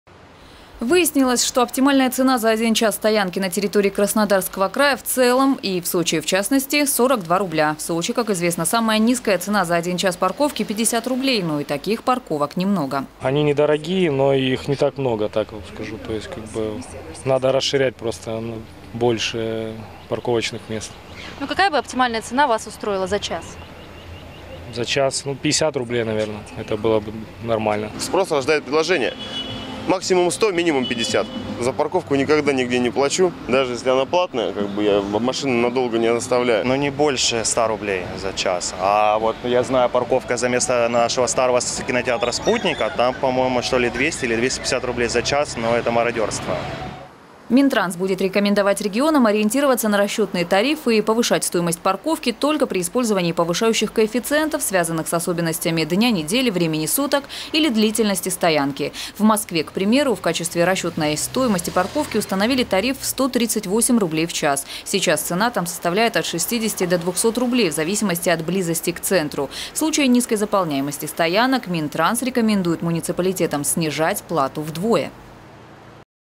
Минтранс рассчитал оптимальную цену на услуги платных стоянок в Сочи. Новости Эфкате Сочи
Выяснилось, что оптимальная цена за 1 час стоянки на территории Краснодарского края в целом и Сочи в частности — 42 рубля. В Сочи, как известно, самая низкая цена за 1 час парковки — 50 рублей, но и таких парковок немного.

Минтранс будет рекомендовать регионам ориентироваться на расчетные тарифы и повышать стоимость парковки только при использовании повышающих коэффициентов, связанных с особенностями дня недели, времени суток или длительности стоянки. В Москве в качестве расчетной стоимость парковки установили тариф в 138 рублей в час. В настоящее время цена там составляет от 60 до 200 рублей, в зависимости от близости к центру. В случае низкого уровня заполняемости стоянок Минтранс рекомендует муниципалитетам снижать плату вдвое или вовсе ее отменить.